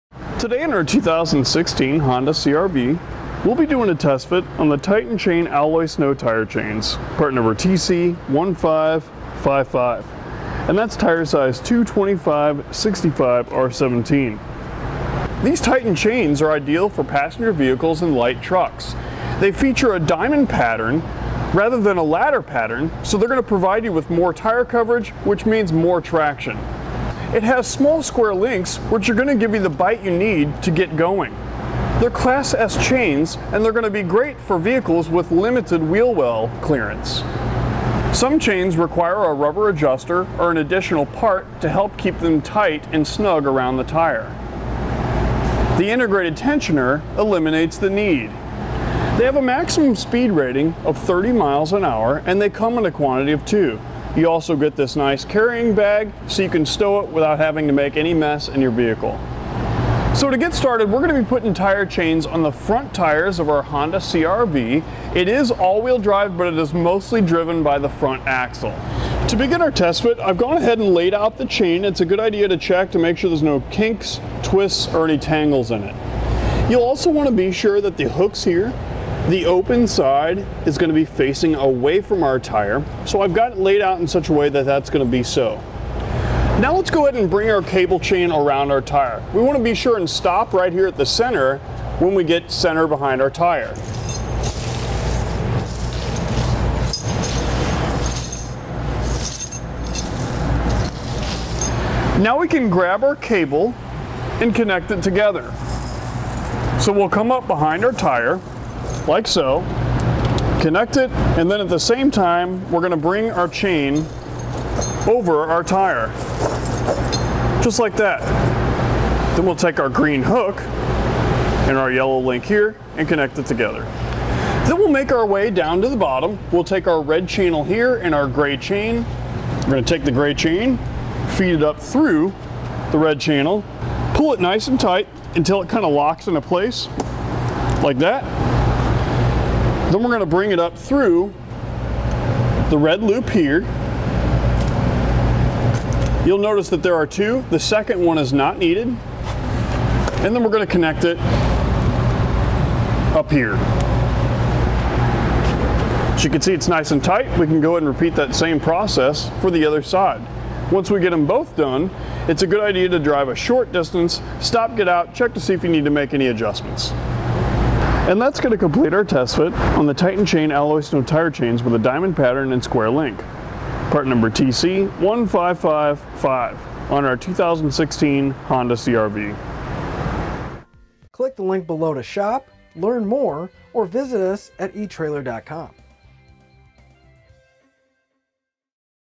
etrailer | How to Install: Titan Chain Snow Tire Chains on your 2016 Honda CR-V
Today on our 2016 Honda CR-V, we'll be doing a test fit on the Titan Chain Alloy Snow Tire Chains, part number TC1555, and that's tire size 225/65/R17. These Titan chains are ideal for passenger vehicles and light trucks. They feature a diamond pattern rather than a ladder pattern, so they're going to provide you with more tire coverage, which means more traction. It has small square links, which are going to give you the bite you need to get going. They're Class S chains, and they're going to be great for vehicles with limited wheel well clearance. Some chains require a rubber adjuster or an additional part to help keep them tight and snug around the tire. The integrated tensioner eliminates the need. They have a maximum speed rating of 30 miles an hour and they come in a quantity of 2.

You also get this nice carrying bag so you can stow it without having to make any mess in your vehicle. To get started, we're going to be putting tire chains on the front tires of our Honda CR-V. It is all-wheel drive, but it is mostly driven by the front axle. To begin our test fit, I've gone ahead and laid out the chain. It's a good idea to make there's no kinks, twists, or any tangles in it. You'll also want to be sure that the hooks here, the open side, is going to be facing away from our tire, so I've got it laid out in such a way that that's going to be so. Now, let's go ahead and bring our cable chain around our tire.

We want to be sure and stop right here at the center when we get center behind our tire. Now, we can grab our cable and connect it together. We'll come up behind our tire like so, connect it, and then at the same time, we're going to bring our chain over our tire just like that. Then, we'll take our green hook and our yellow link here and connect it together. Then, we'll make our way down to the bottom. We'll take our red channel here and our gray chain.

We're going to take the gray chain, feed it up through the red channel, pull it nice and tight until it locks into place like that. Then, we're going to bring it up through the red loop here. You'll notice that there are 2. The second one is not needed, and then we're going to connect it up here. As you could see it's nice and tight.

We can go ahead and repeat that same process for the other side. Once we get them both done, it's a good idea to drive a short distance, stop, get out, check to see if you need to make any adjustments. That's going to complete our test fit on the Titan Chain Alloy Snow Tire Chains with Diamond Pattern and Square Link, part number TC1555, on our 2016 Honda CR-V.